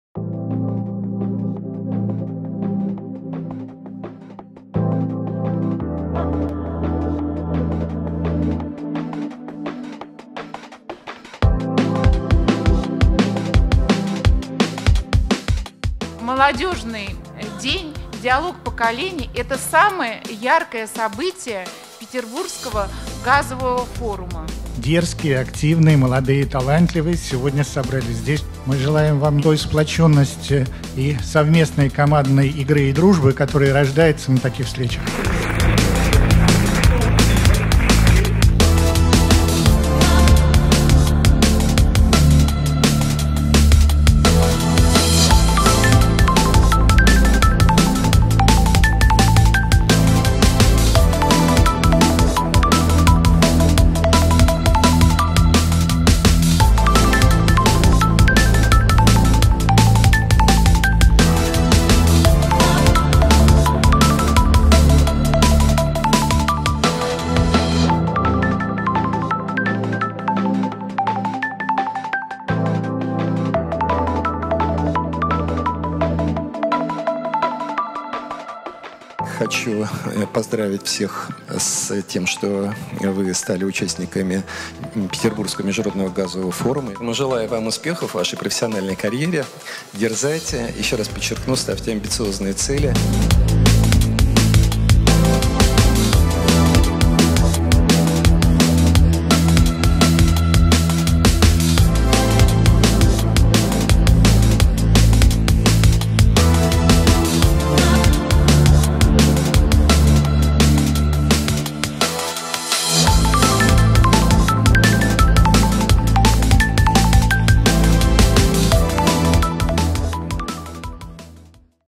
Молодежный день на ПМГФ-2018 / Youth day SPIGF 2018
Молодежный день – уникальная информационная интеллектуальная и полемическая площадка, призванная объединить представителей отраслевой бизнес-элиты и молодежную аудиторию ведущих университетов и институтов мира для обсуждения новых векторов развития газовой отрасли и карьерных возможностей для молодых лидеров.

Организаторы

Высшая экономическая школа Санкт-Петербургского государственного экономического университета
Фонд международных инновационных программ
Партнеры

Международный деловой конгресс (МДК)
ПАО «Газпром»
Компания «Uniper»
Компания Wintershall:
Компания Schneider Electric
Институт «Энергетическая Дельта»
Молодежный день проводится при финансовой и организационной поддержке Международного делового конгресса (МДК) и включён в официальную программу VII Петербургского Международного Газового Форума.

Участники Молодёжного дня – талантливые студенты из разных стран мира, влиятельные эксперты и молодые лидеры крупнейших международных энергетических компаний и представители академического сообщества.

Уникальные преимущества проекта

Открытый диалог поколений
Передача знаний и опыта
Моделирование реальных ситуаций и учет отраслевой специфики
Вовлеченность талантливой молодежи
Глобальное видение и кросс-культурное взаимодействие
Принцип социальной ответственности и устойчивого развития
Программа «Молодежного дня» включает различные мероприятия, проводимые в интерактивном или соревновательном формате. Участники проведут серию встреч и дискуссий на различные темы. Особое внимание будет уделено карьерным возможностям и привлекательности отрасли с точки зрения студентов.

2 октября состоится открытие Молодежного дня, и пройдут интерактивные work-shops, организованные компаниями-партнерами Молодежного дня.

3 октября – кейс-турнир, в котором примут участие 5-6 команд студентов, отобранных на предварительном этапе.

4 октября запланировано проведение закрытого мероприятия «Встреча без галстуков», на котором перед участниками Молодежного дня выступят CEO ведущих энергетических компаний.